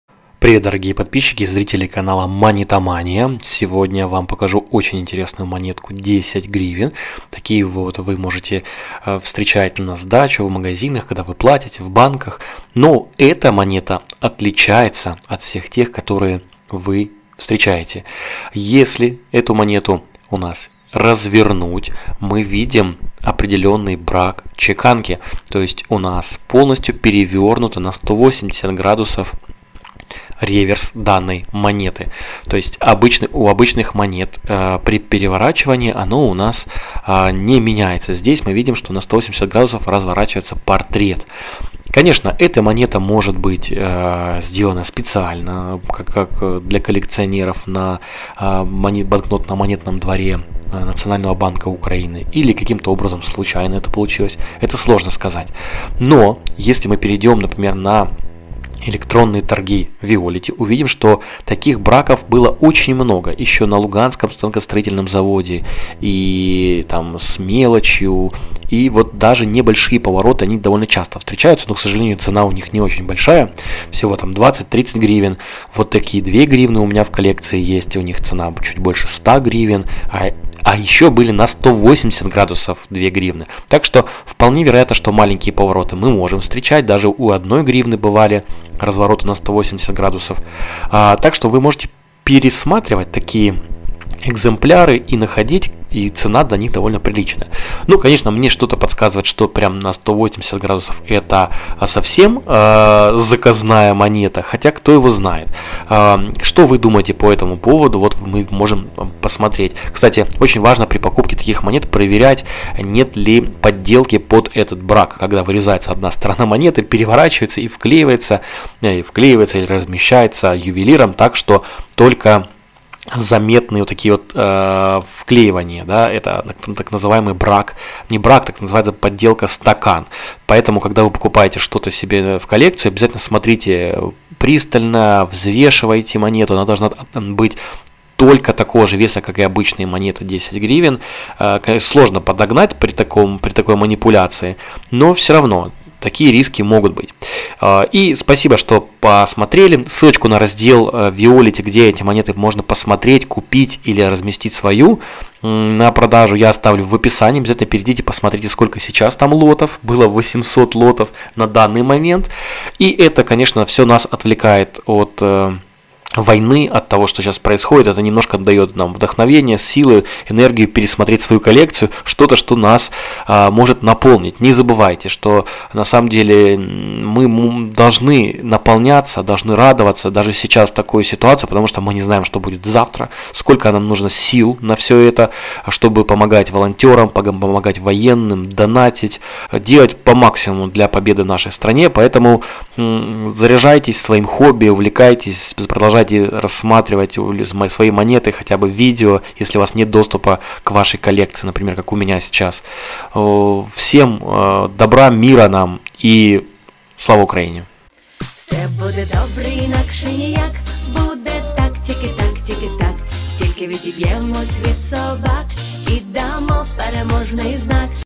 НЕ СДАВАЙТЕ НОВЫЕ 10 ГРИВЕН МОНЕТОЙ! ПОКА НЕ ПРОВЕРИТЕ! Отложите дорогие 10 ГРН 2020 ГОДА
В  видео Вы увидите самые дорогие и редкие из новых МОНЕТ Украины. Новая одна гривна 2018,  новые ДВЕ ГРИВНЫ 2018 , 5 гривен 2019 монетой и 10 гривен монетой 2020 года.

Сейчас во время войны с Россией эти монеты не утратили своей стоимости их покупают и продают
Зайдите и посмотрите цены.

❓О канале МОНЕТОМАНИЯ: канал про редкие и дорогие монеты Украины и не только. О том как можно заработать на монетах.